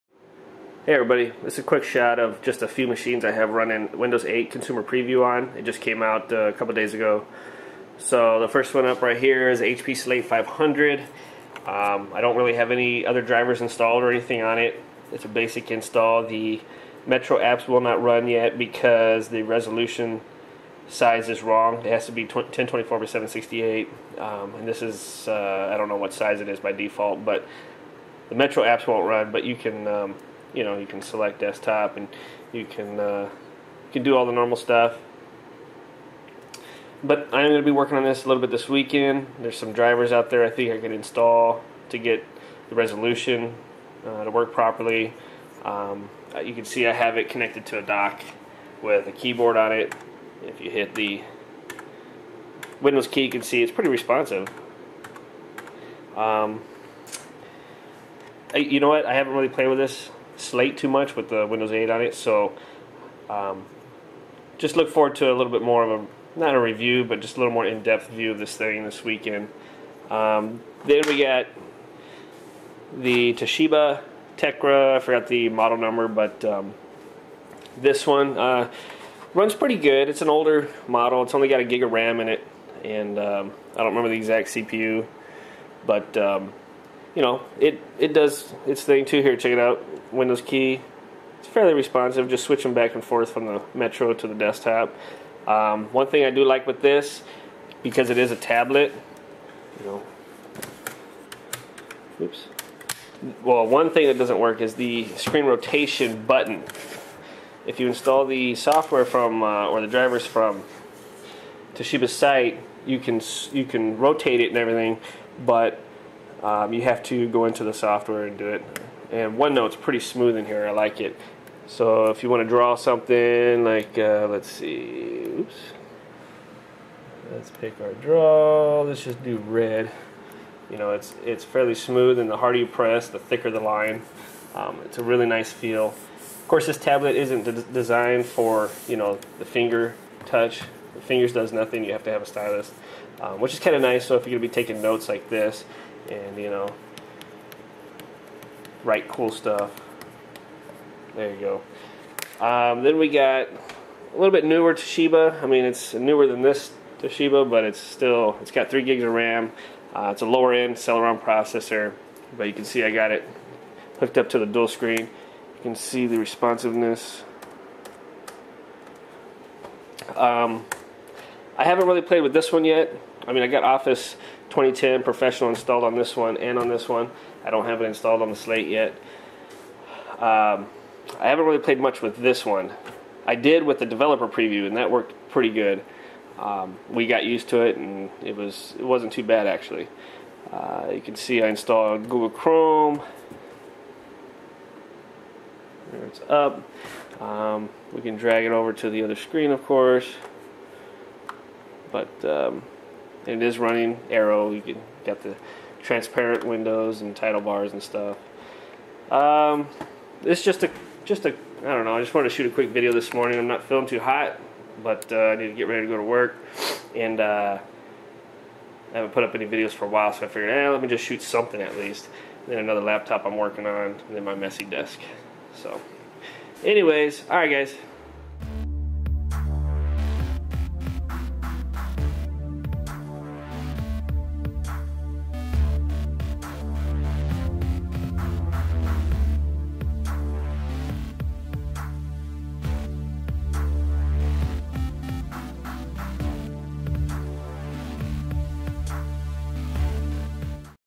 HP Slate and More | Windows 8 Consumer Preview | Quick Look
Quick look at Windows 8 Consumer Preview on an HP Slate 500, Toshiba laptop (LS355-S7915), Toshiba tablet (Tecra M4-S435).